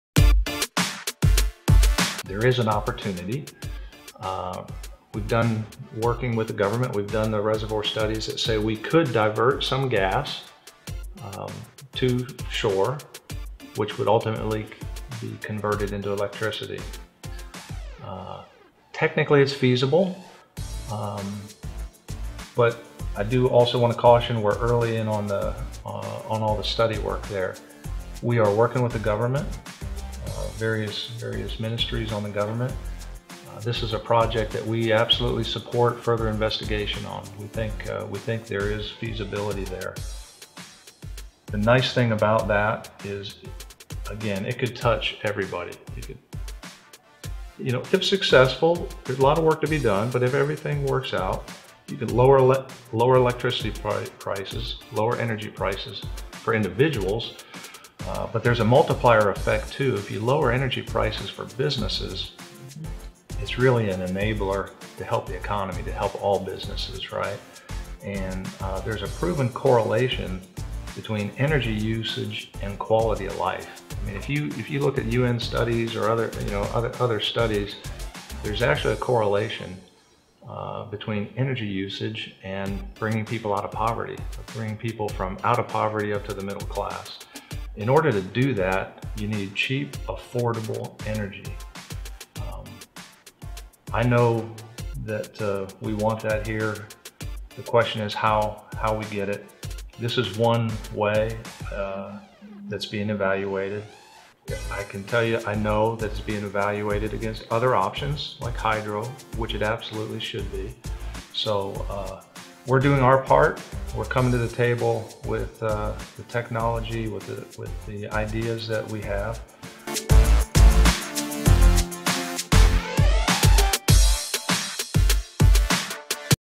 Diverting natural gas to shore technically feasible
ExxonMobil Country Manager, Rod Henson, says based on preliminary investigations, diverting natural gas to shore in Guyana is technically feasible, but the study of making this a reality is still in the early stage.s